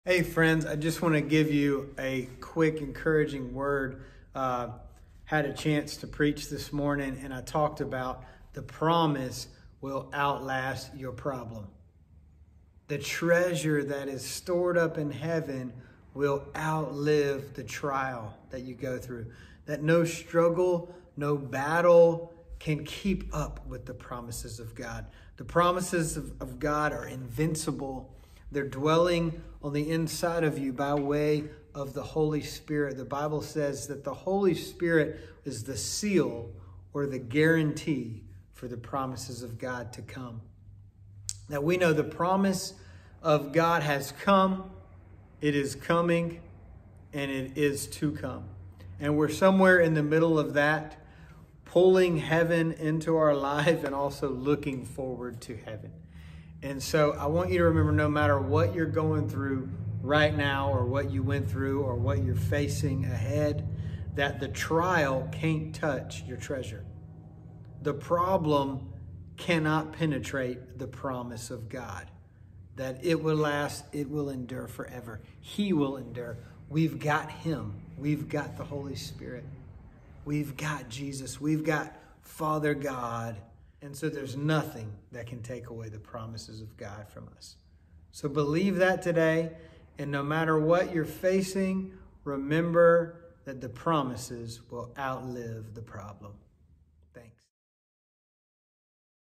The Promise Outlives the Problem
A short recap of a message I had the opportunity to share recently about the promises of God and how the always outlast the problems. We have a hope that does not disappoint!